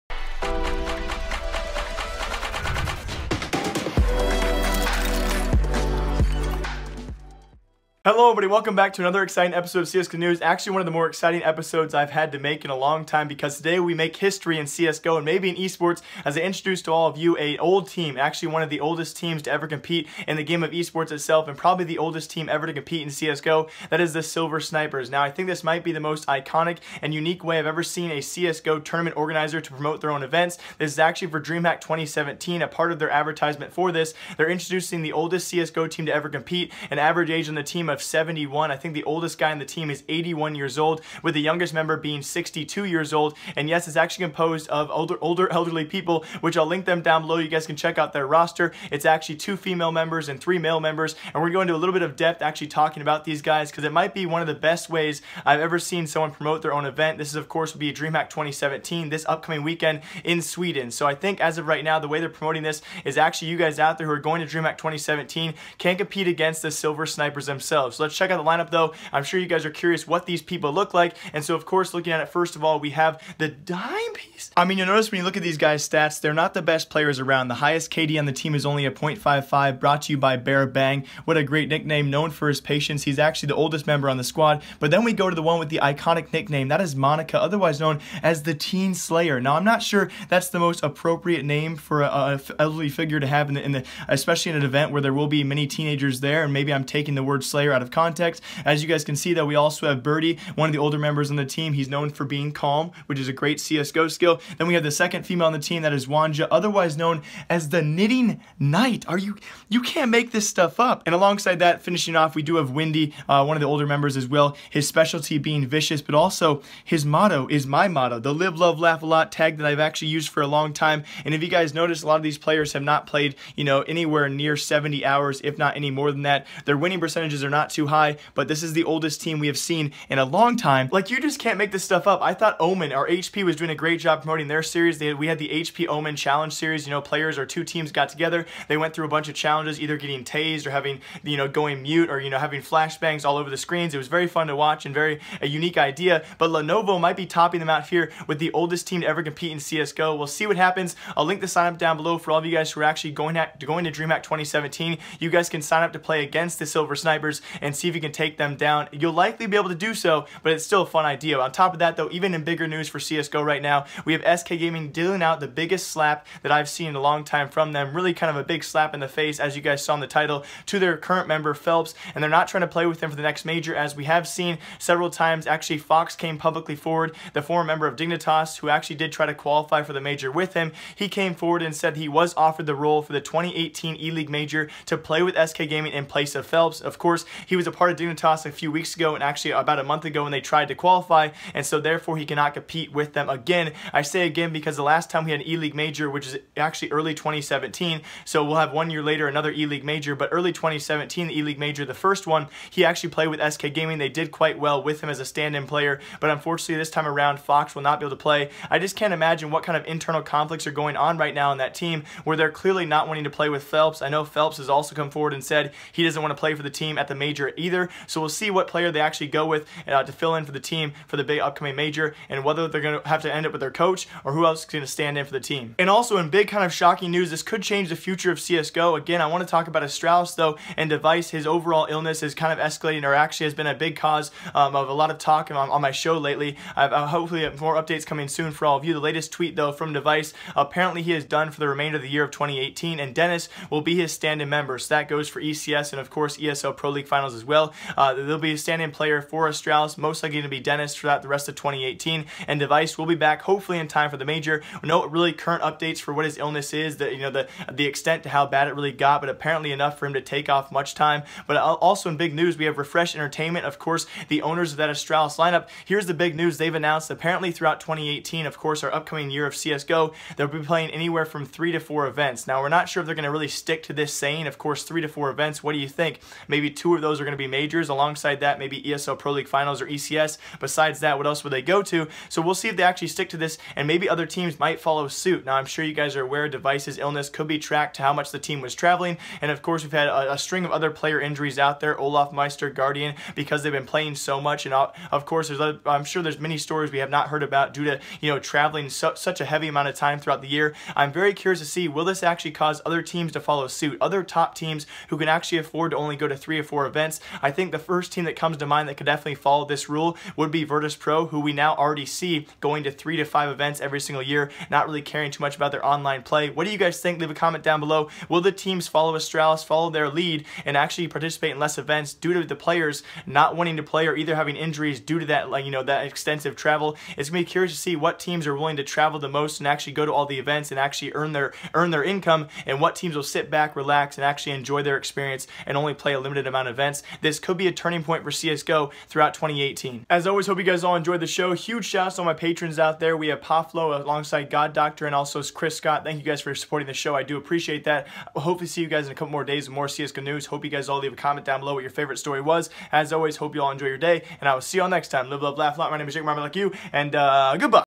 Meet the Oldest Team in CSGO History! SK Slap Felps in the Face and Astralis Could Change EVERYTHING
1:15 - Meet the Oldest CSGO Players
2:11 - One of the Best Promotions We Have Seen
2:41 - SK Gaming Looking for Subs for Felps
3:50 - Dev1ce Updates - Done for 2018
4:22 - RFRSH Announce FEWER Events in 2018